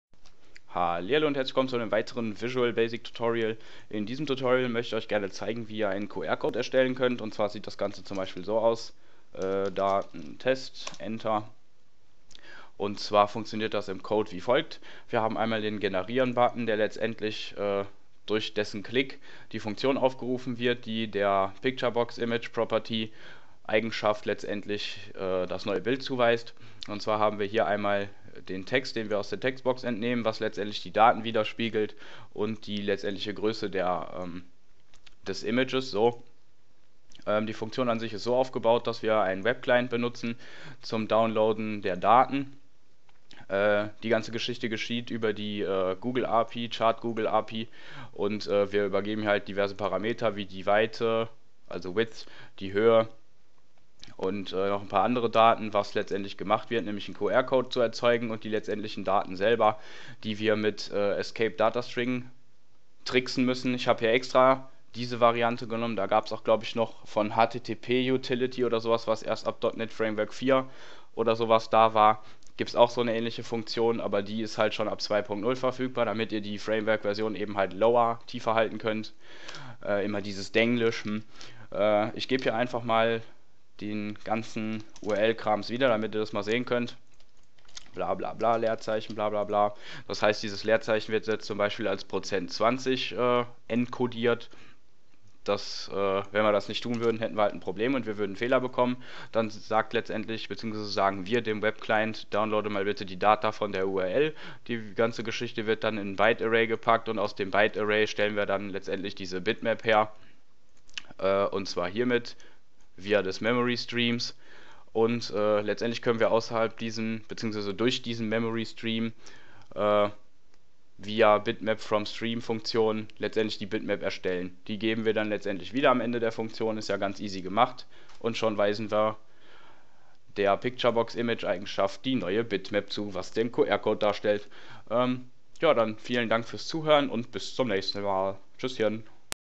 VB.NET QR Code erzeugen/generieren (ohne lokale externe Lib/dll) [HowTo #032]
BESCHREIBUNG
In diesem Video zeige ich euch wie Ihr mithilfe der Google Chart API einen Barcode erzeugen könnt.